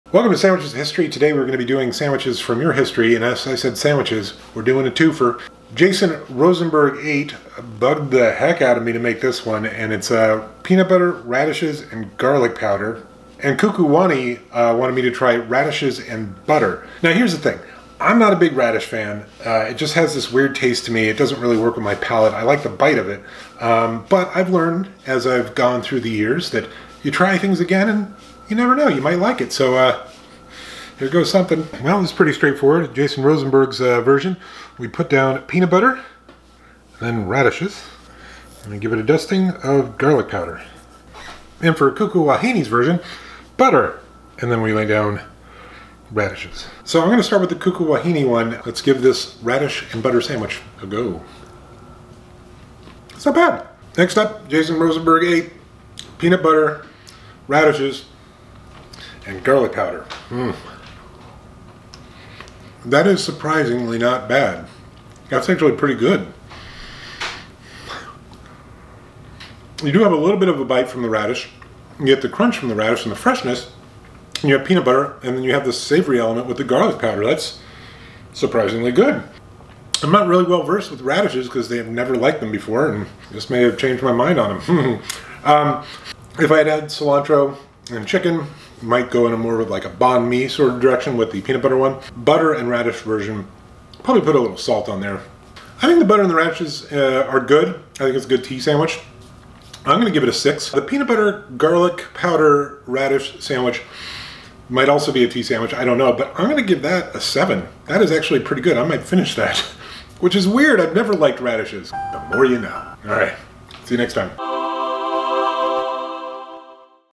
Radish Sandwiches on Sandwiches of History⁣ (Our History)
So, yeah, time to do some sandwiches that, if I’m honest, I’ve been putting off. Mainly because I‘m not a fan of the main ingredient - radishes. @jasonrosenberg8 (TikTok) wanted to try a sandwich made with radishes, peanut butter, and garlic powder while @kukuwahini (TikTok)wanted me to give radishes and butter a go. So I figure, Imma jump into the deep end of the pool and do both in one go.
Get sneak peeks, exclusive vids, merch, and monthly livestreams, and more!